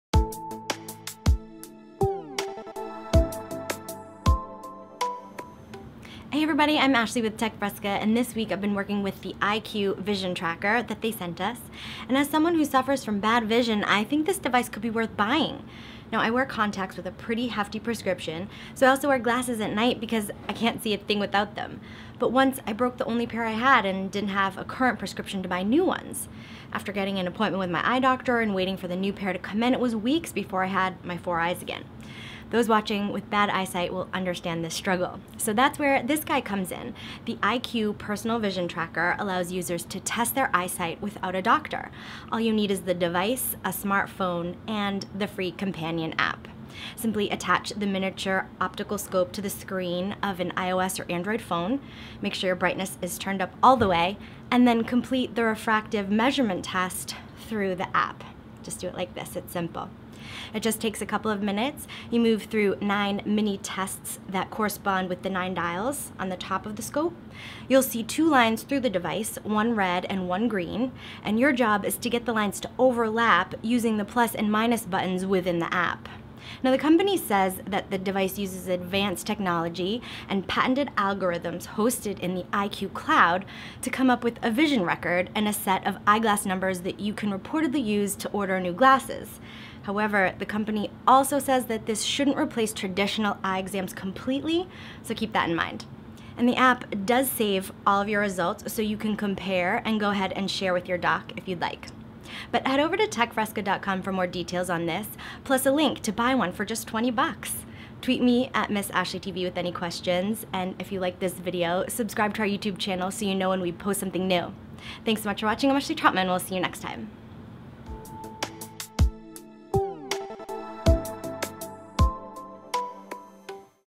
Test Your Own Vision With the EyeQue
The EyeQue Personal Vision Tracker allows users to test their eyesight without a doctor present. More details are up now on techfresca.com.